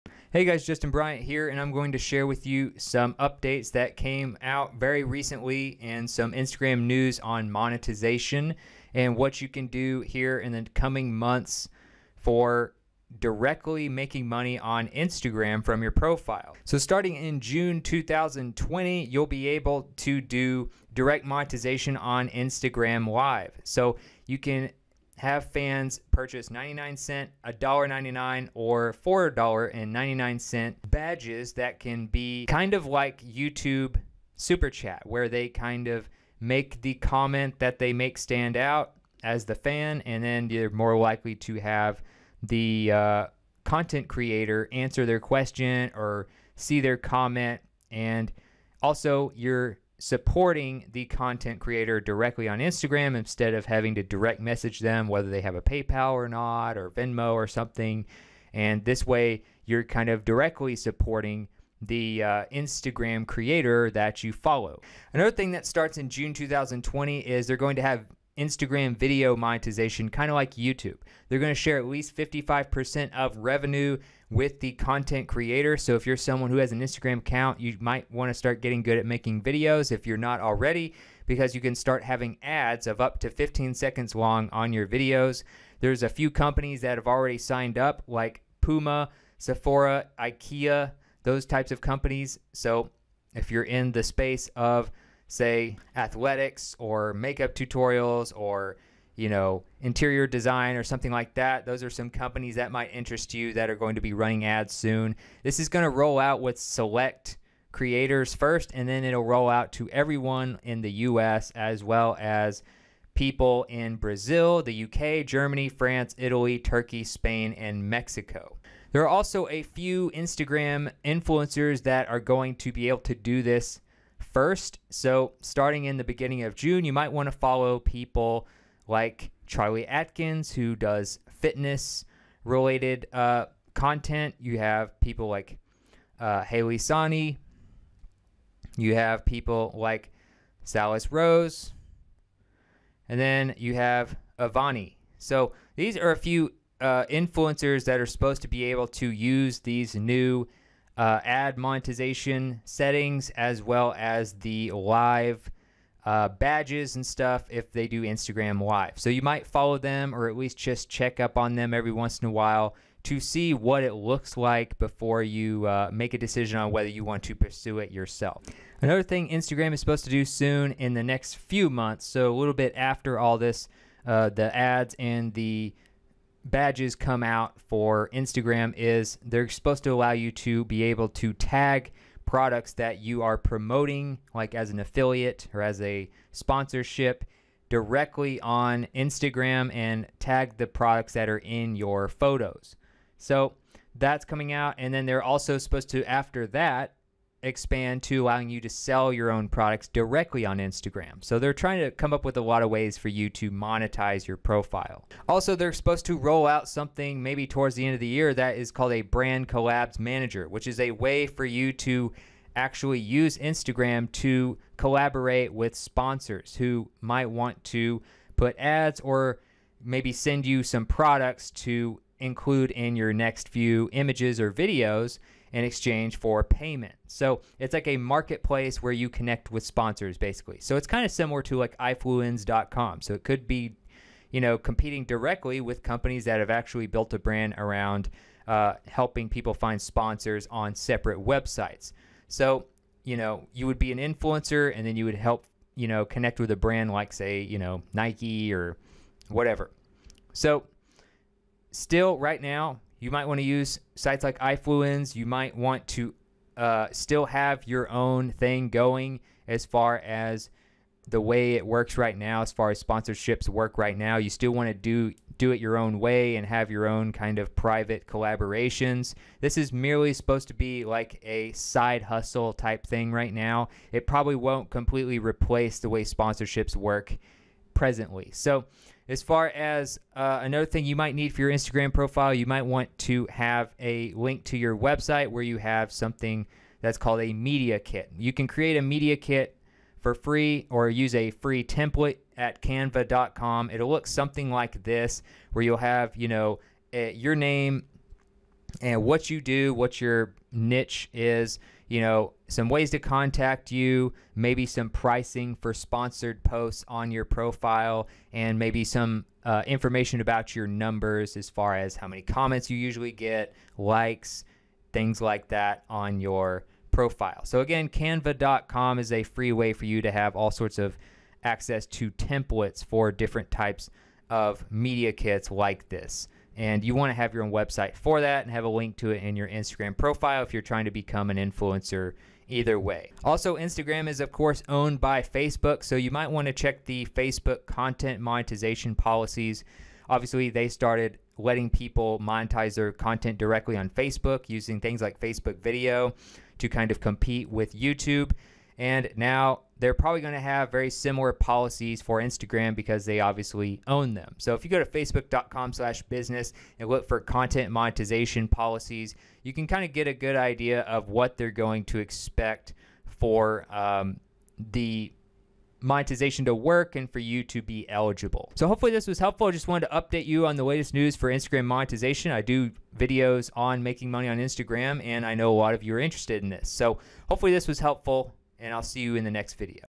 INSTAGRAM NEWS: Instagram Pays You Directly with New Monetization Policies
Here's some BIG news from Instagram that talks about 3 or 4 new ways to monetize your profile directly. for video notes, related content, tips, and helpful resources mentioned.